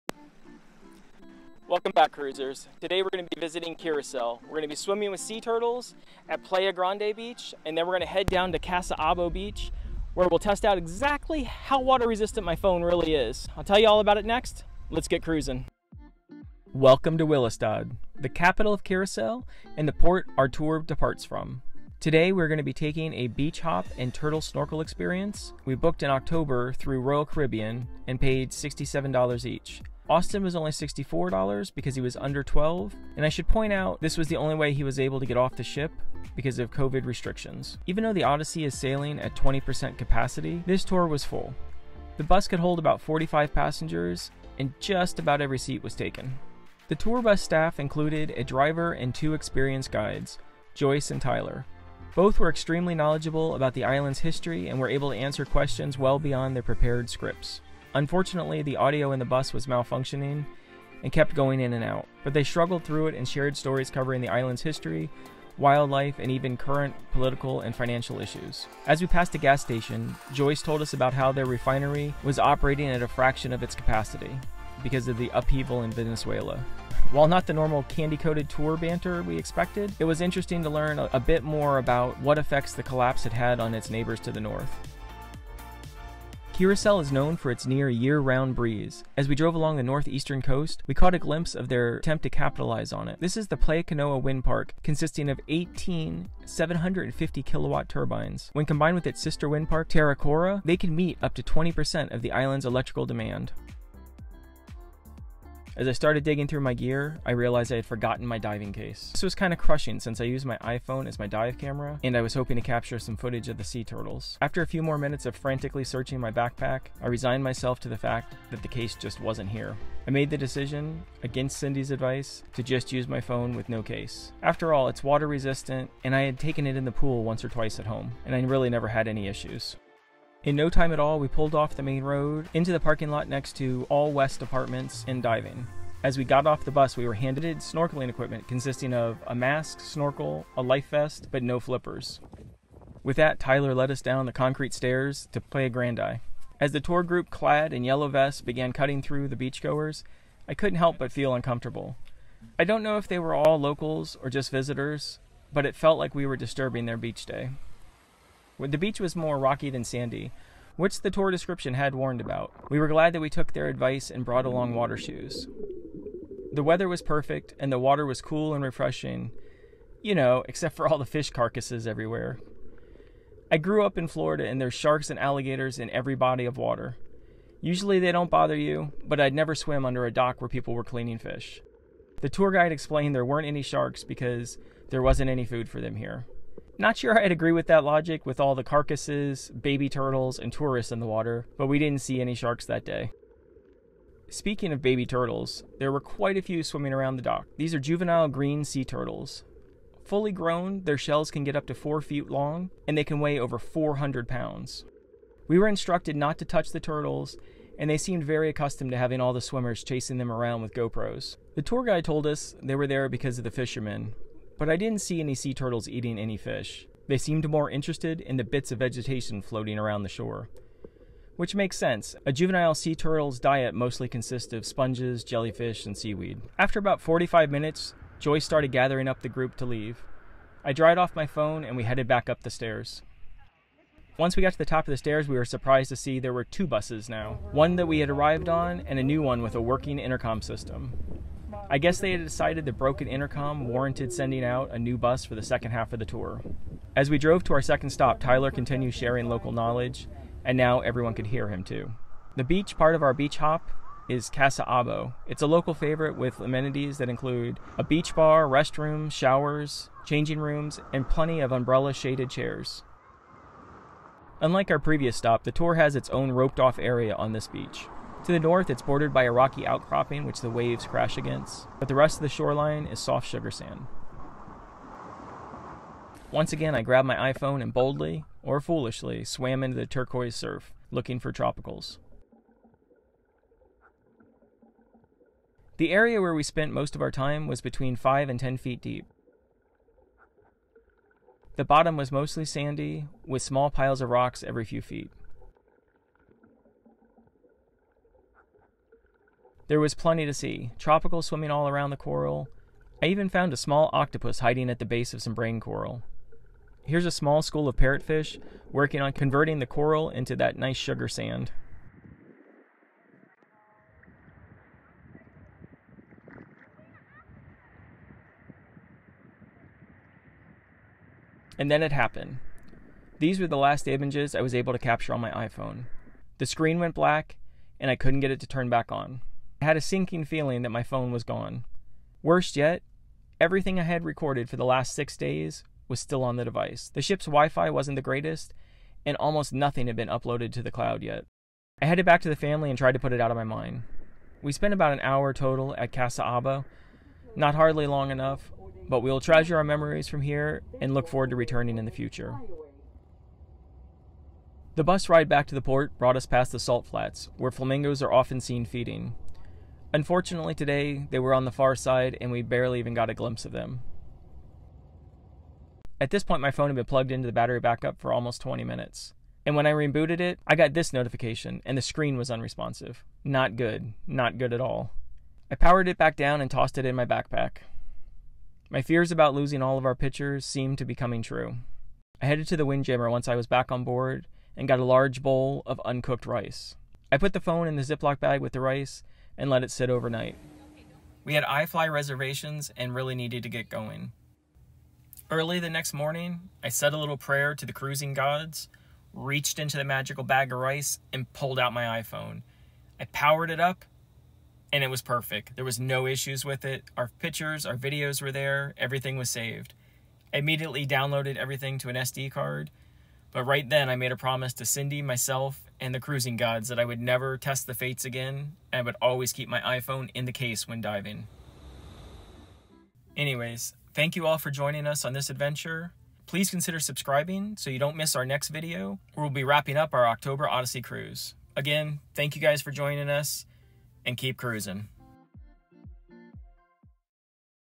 ROYAL CARIBBEAN AMAZING SNORKELING IN CURAÇAO
Royal Caribbean Odyssey of the Seas cruise visit to Curacao. Today’s excursion is called “Beach Hop & Turtle Snorkel Experience” we booked it through Royal Caribbean and because of Covid restrictions booking a Royal Caribbean and begins with boarding the tour bus that was waiting for us at the end of the pier. We traveled north on Weg Naar Westpunt, a narrow two lane road through small villages, and beautiful countryside while the tour guides shared stories about Curacao’s history. Our first stop is Playa Grandi, a small rocky beach that’s apparently quite popular with juvenile green sea turtles. Then we head to Cas Abao Beach, it’s a local favorite that features a beach bar and plenty of umbrella shaded beach chairs. To the north the sugar sand beach ends at a natural rock formation that the surf crashes into, this location was ready made for postcards from paradise. Later we pass the salt flats hoping for a glimpse of Curacao’s flamingos.

Also on this adventure I forget the dive case for my iPhone and decide to test its water resistance 📱🌊😓

  Your support is appreciated and we hope you enjoy our videos!